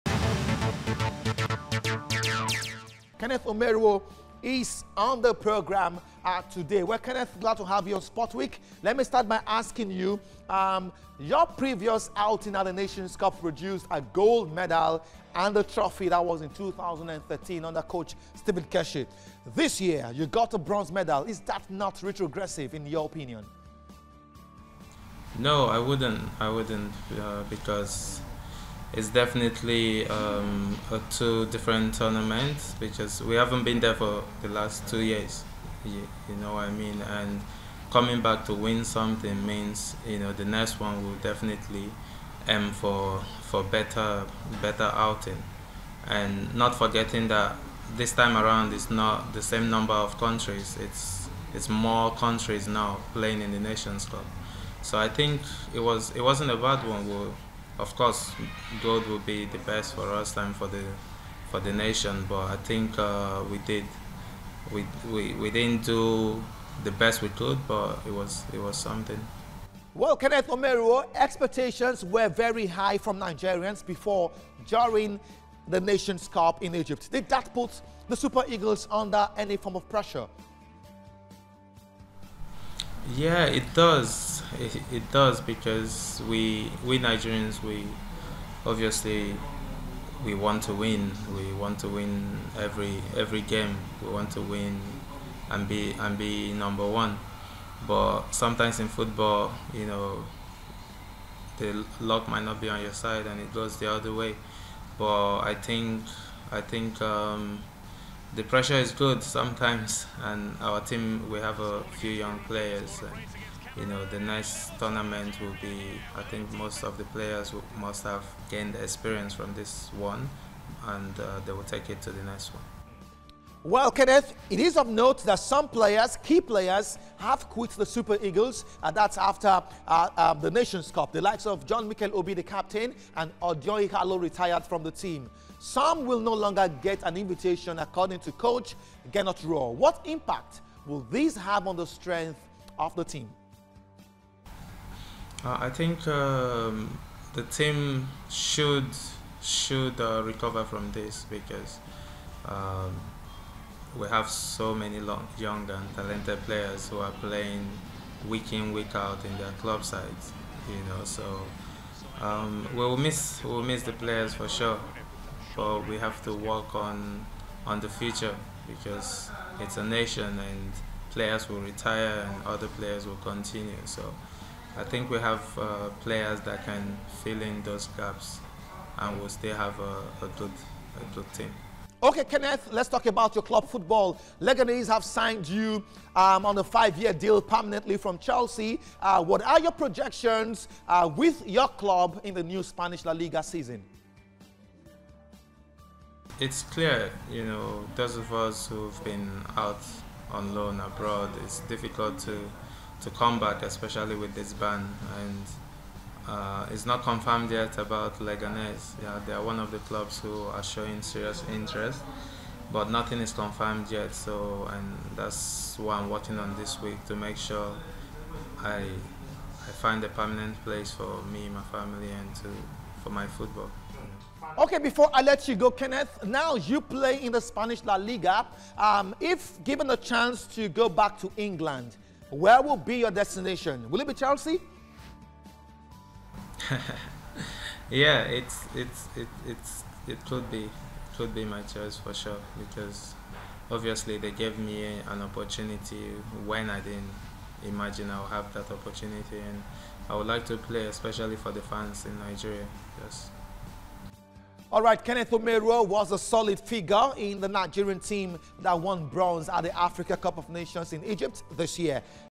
Super Eagles defender Kenneth Omeruo speaks on AFCON 2021
kenneth says his previous outing at the nation's cup producing a bronze medal is not retrogresive because they are two different tournaments.